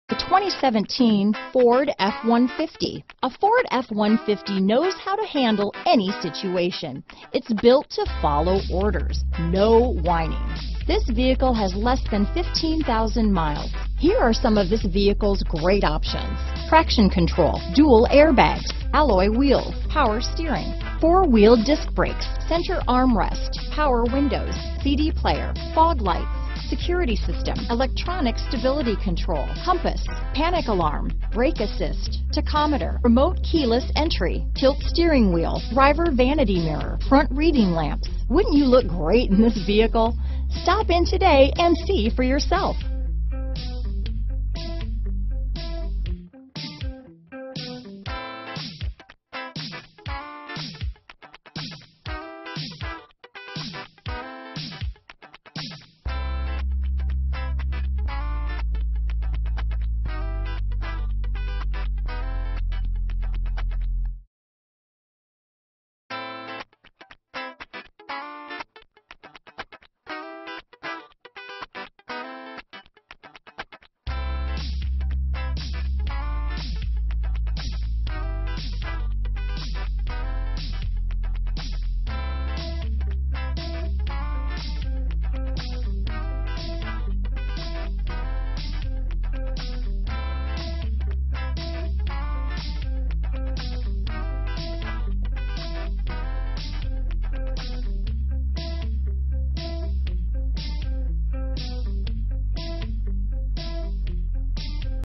2017 Ford F-150 at Oxmoor Hyundai | Louisville & Lexington, KY H11771A
Lightning Blue U 2017 Ford F-150 available in Louisville, Kentucky at Oxmoor Hyundai. Servicing the Lexington, Elizabethtown, KY, New Albany, IN, Jeffersonville, IN area.

Oxmoor Hyundai
8107 Shelbyville Rd

Ford F-150 XLT CARFAX One-Owner. ALLOY WHEELS, 1-OWNER, BLUE TOOTH TECHNOLOGY, VEHICLE SERVICED AND DETAILED, LOCAL TRADE!!, STEERING WHEEL MOUNTED RADIO CONTROLS, F-150 XLT, 4D SuperCrew, 5.0L V8 FFV, 6-Speed Automatic Electronic, 4WD, Lightning Blue, Black w/Unique Sport Cloth 40/Console/40 Front-Seats, Accent-Color Step Bars, Black Billet Style Grille w/Body-Color Surround, Body-Color Door & Tailgate Handles, Body-Color Front & Rear Bumpers, Box Side Decal, Single-Tip Chrome Exhaust, Wheels: 18 6-Spoke Machined-Aluminum, XLT Sport Appearance Package. Odometer is 13185 miles below market average! 6-Speed Automatic ElectronicbrAwards:br  * 2017 KBB.com Brand Image Awards   * 2017 KBB.com 10 Most Awarded Brands